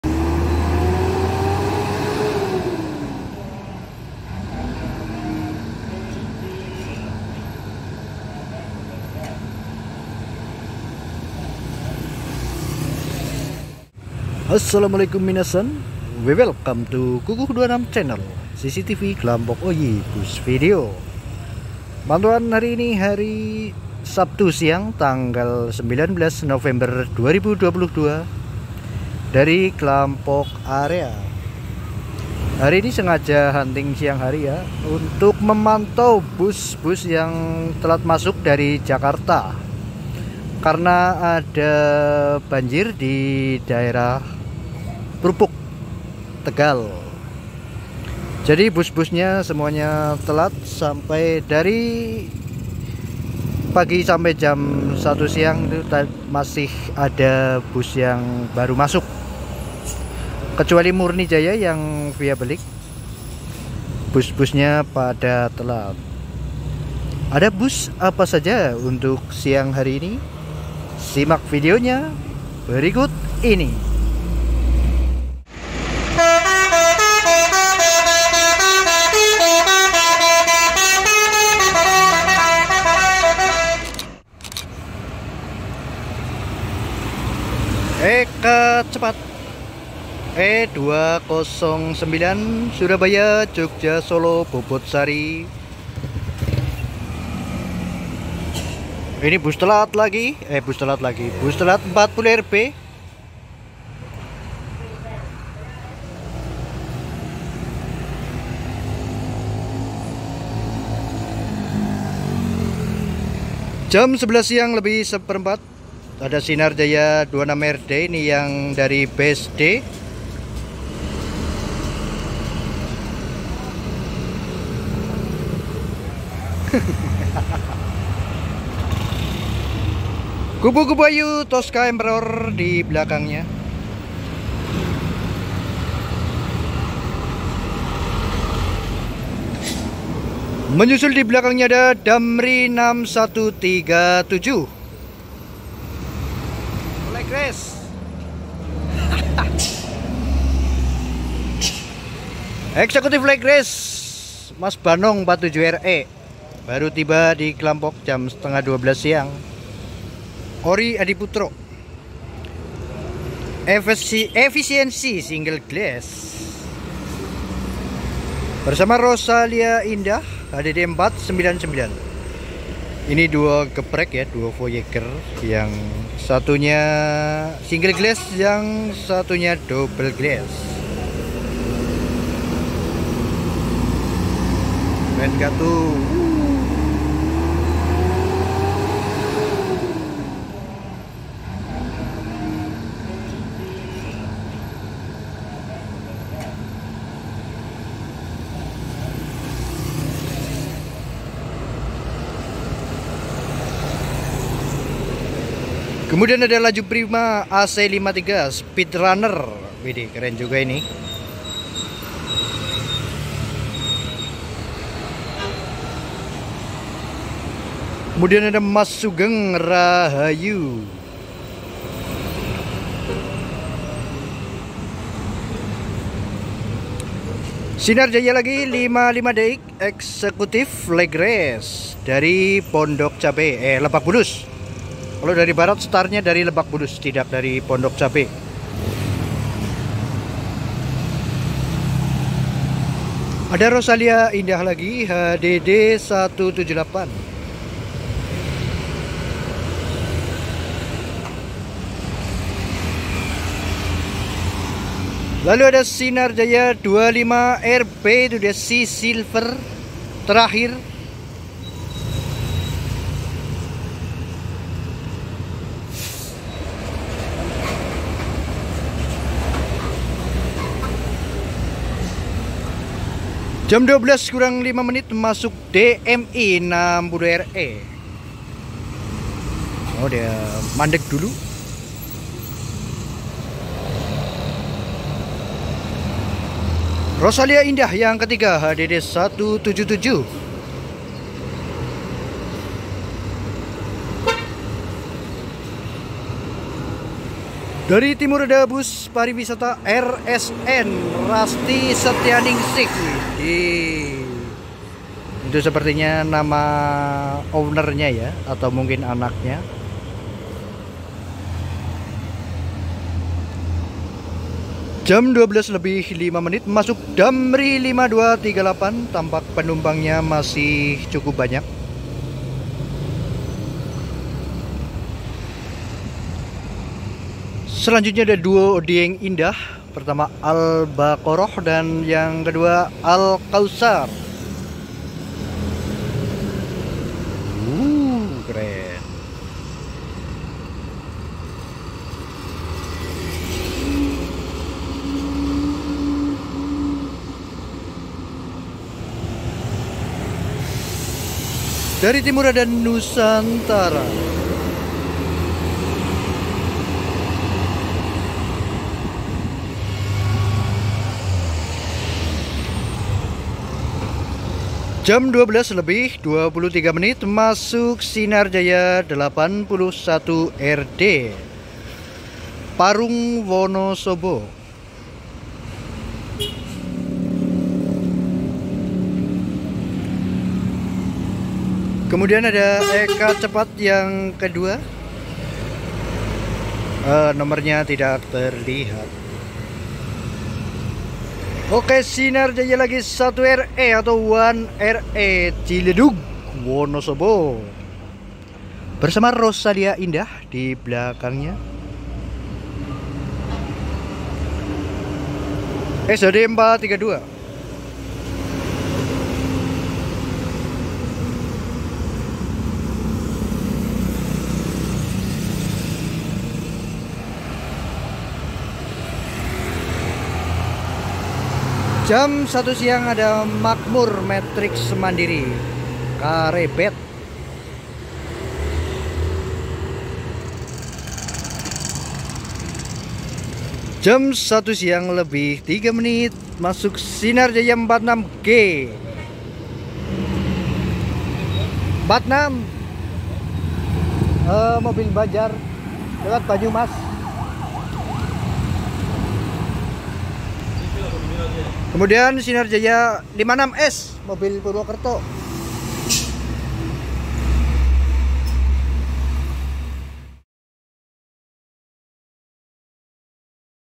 Efek Banjir di Prupuk, Semua Bus Yang Via Bumiayu Kesiangan ‼️
assalamualaikum minasan
selamat datang di kukuh 26 channel

pantauan siang hari dari agen sinar jaya klampok.
banyak bus yang kesiangan sampai klampok, efek banjir di prupuk tadi malam. banyak yang baru masuk antara jam 11 sampai jam 1 siang.